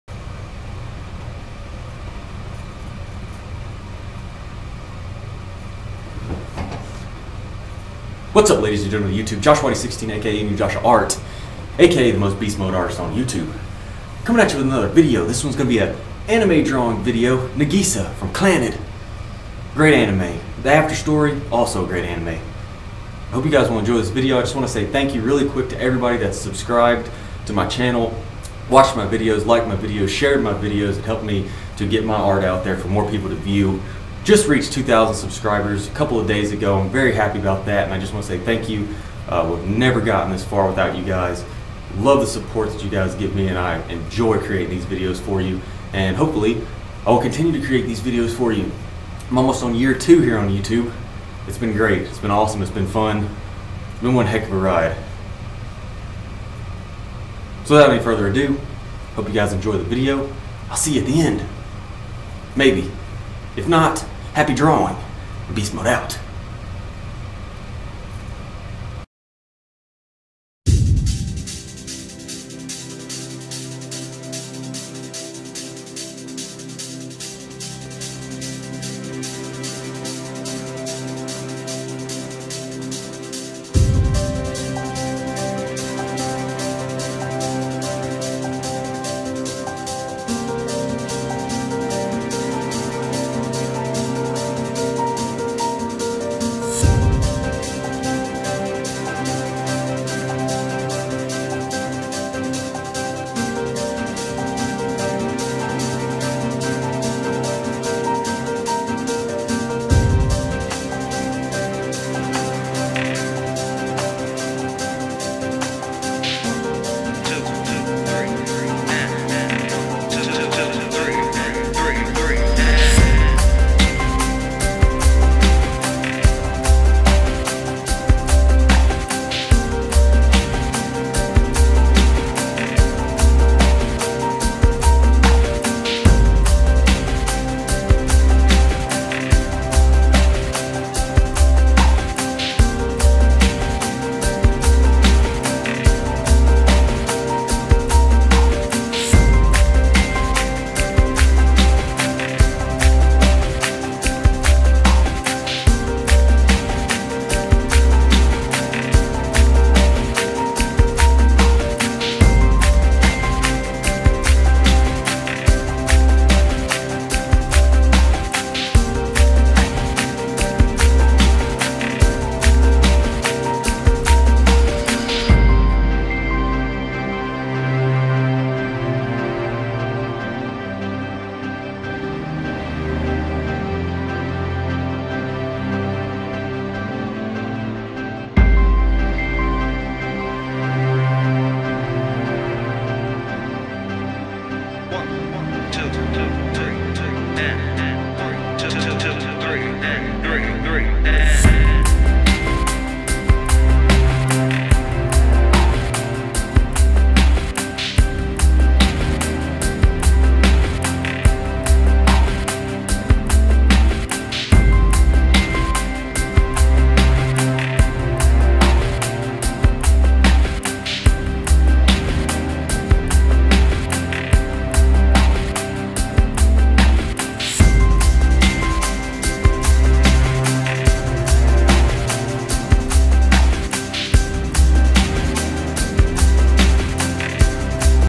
Lets Draw Nagisa Furukawa
whats up guys , im back with another anime drawing lets draw video. going to be drawing nagisa furukawa today , i hope you enjoy it!! Happy Drawing my friends :)